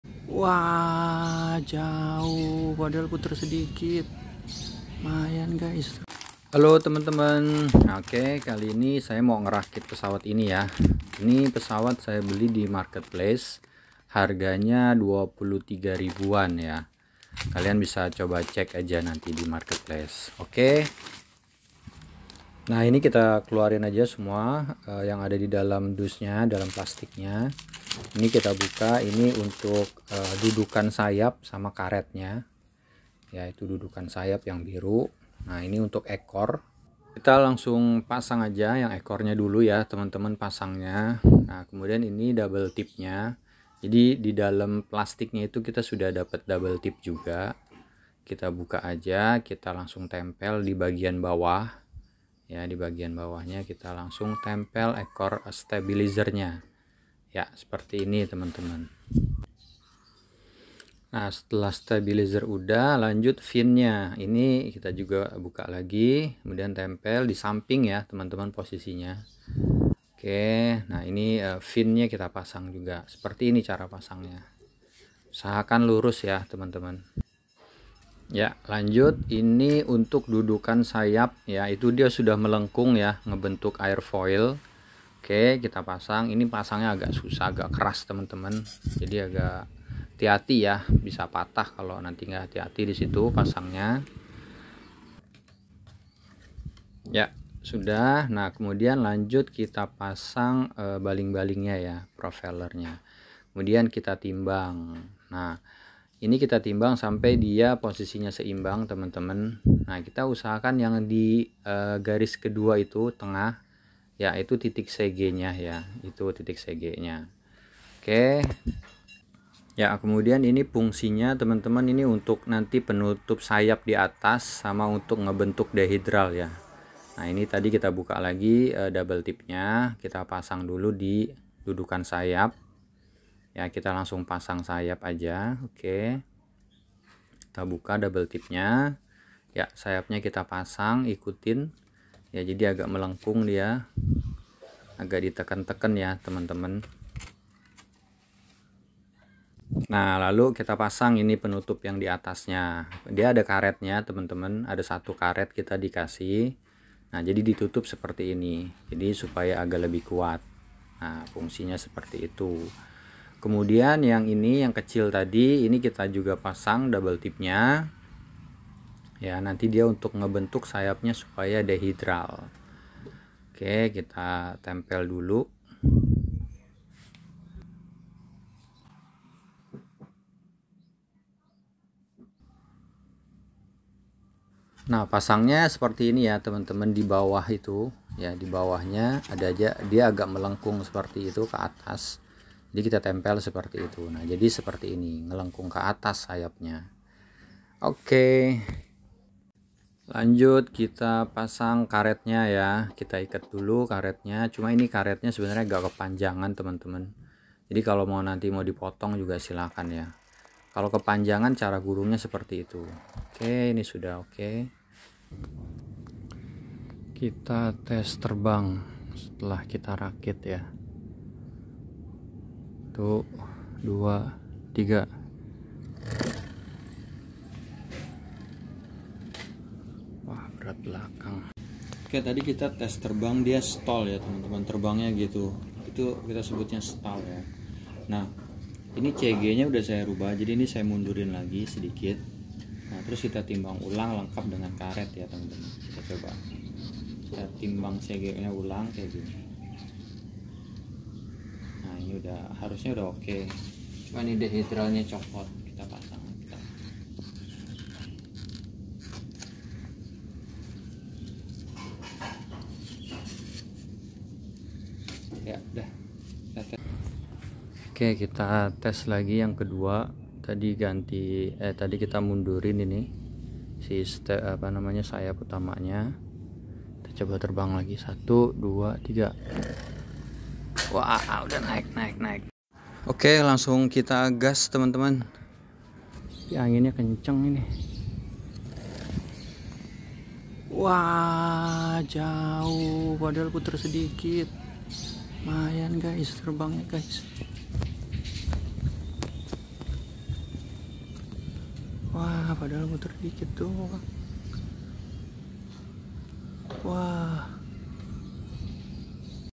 MERAKIT PESAWAT RP 20000an, UNBOXING I BUILDING I FLYING I
Merakit Pesawat Tenaga Karet Rp 20.000an, dan hasilnya terbang dengan indah guys.
 To Make a Rubber Powered Airplane For Beginners (Cara Membuat Pesawat Tenaga Karet untuk Pemula)
Membuat Pesawat Tenaga Karet untuk pemula  dari Stik Eskrim dan Dus Susu
avion à moteur en caoutchouc, रबर पावर प्लेन, 고무 동력 비행기.avión de goma.

terima kasih